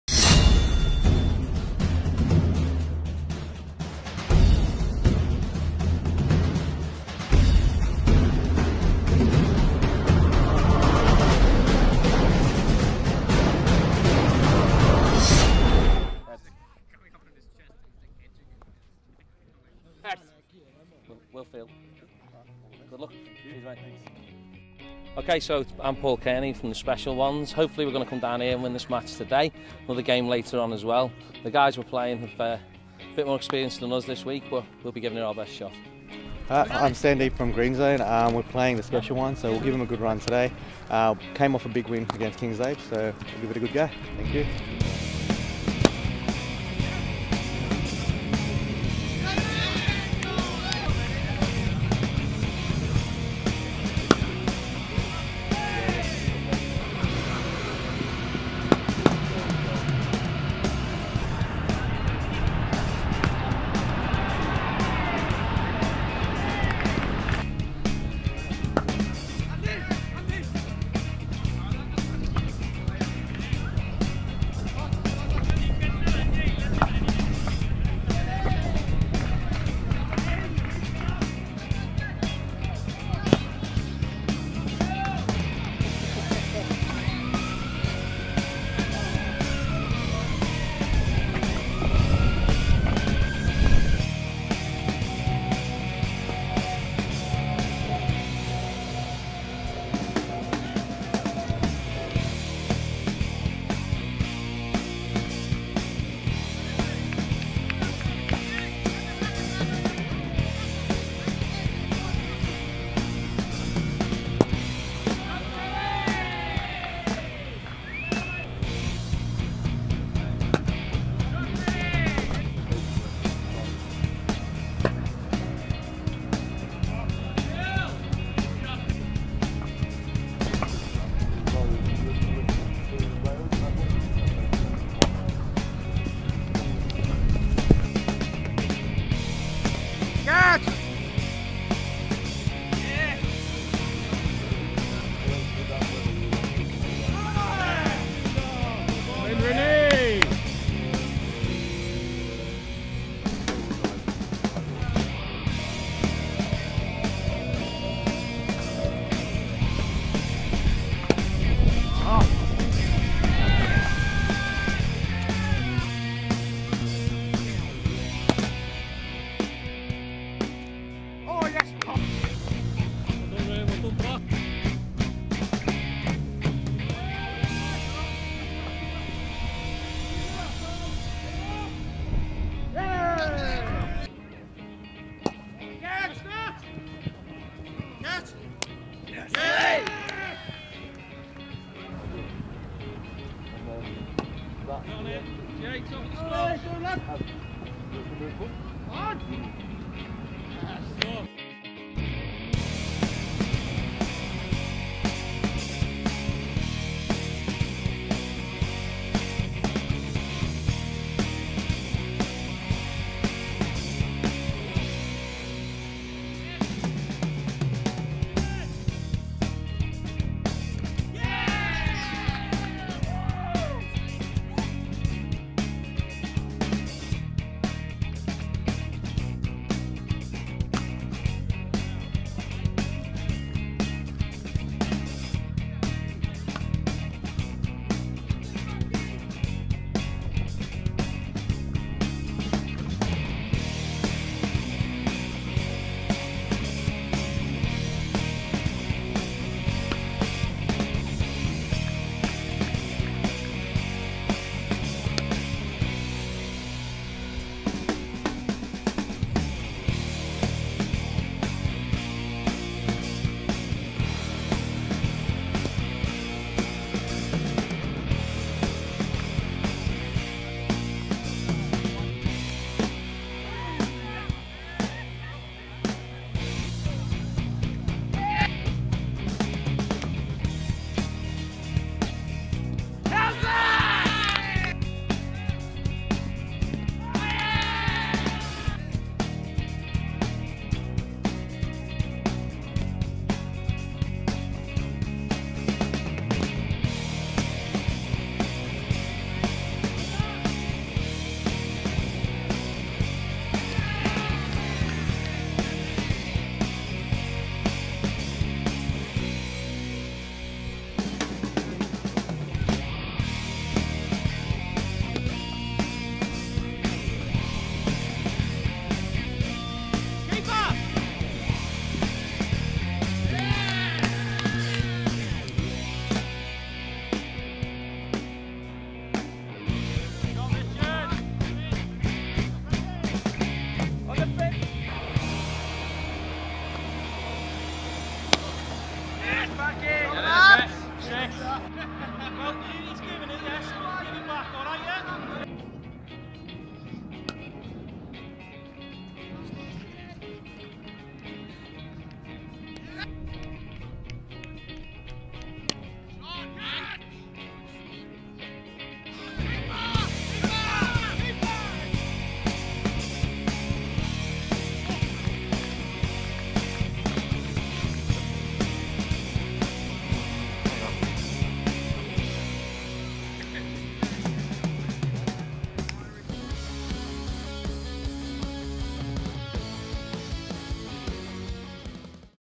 Green Zone vs Special Ones - Last Man Stands World Championships 2013
This Last Man Stands World Championships 2013 match saw Green Zone once again overcome their opponents in a heated match on Dulwich Sports Ground 1.